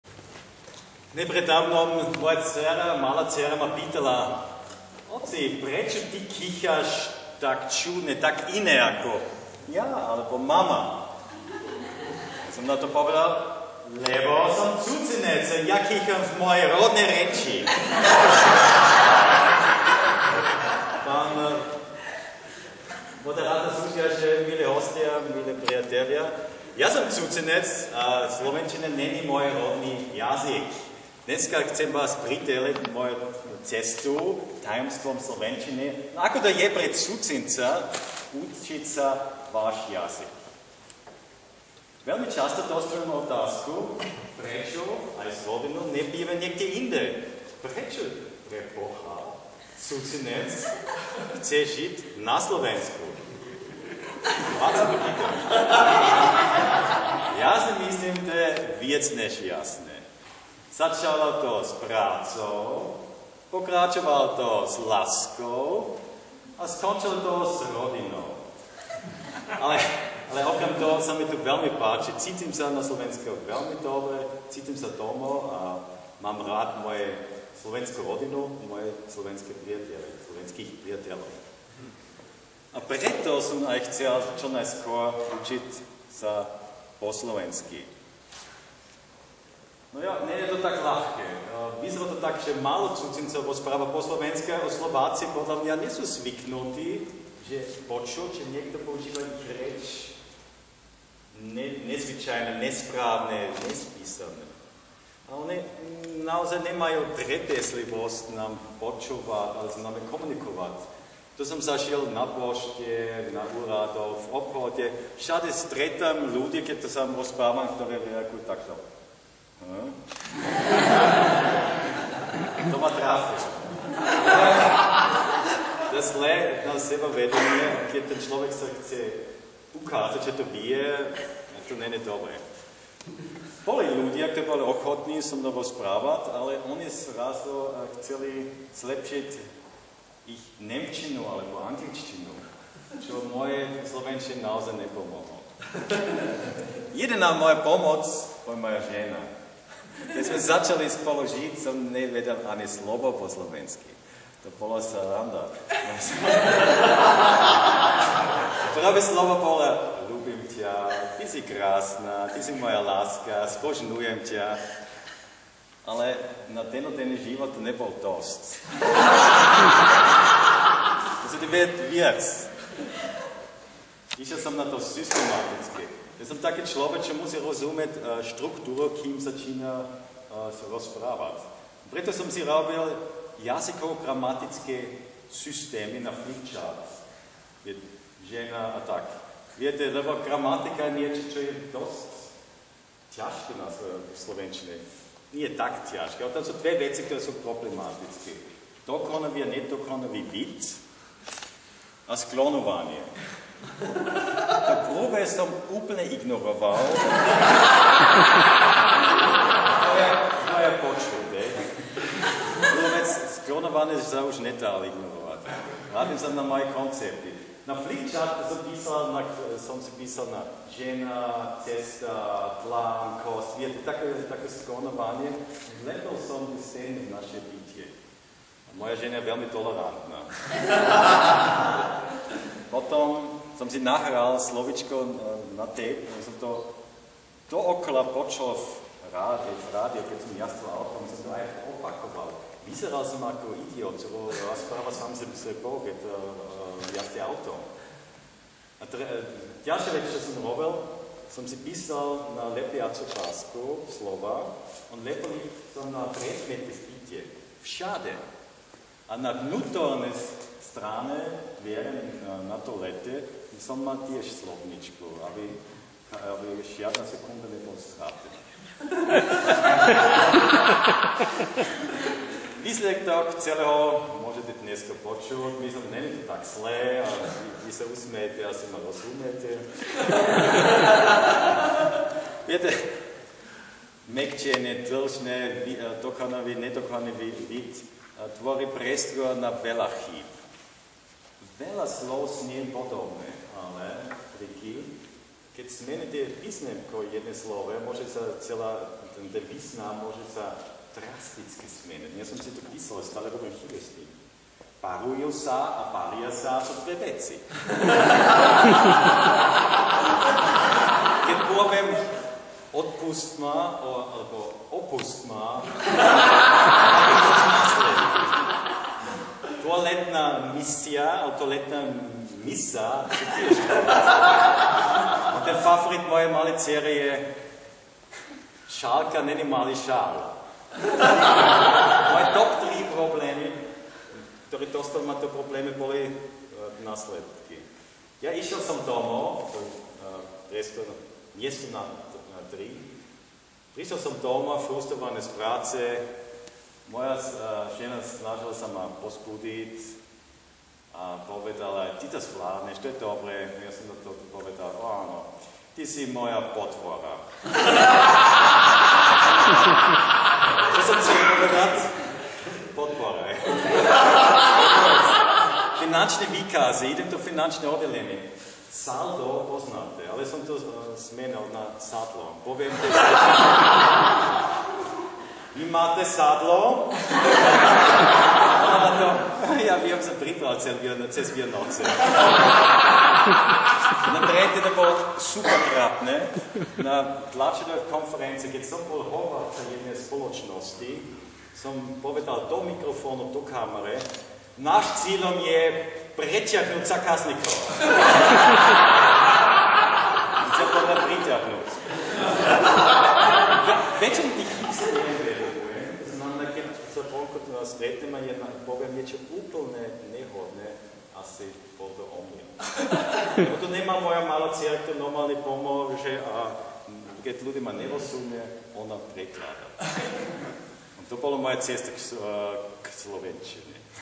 Klaus HAMMER: Cesta k slovenčine (finále)
3. ročník stredoeurópskej rečníckej sútaže Toastmasters, 1. miesto v slovenských pripravených prejavoch, Klaus HAMMER: Cesta k slovenčine (reč vo finále), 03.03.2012, aula Ekonomickej univerzity v Bratislave, kamera a strih: Miloslav OFÚKANÝ, propagácia copyright: GeoCommunity 2012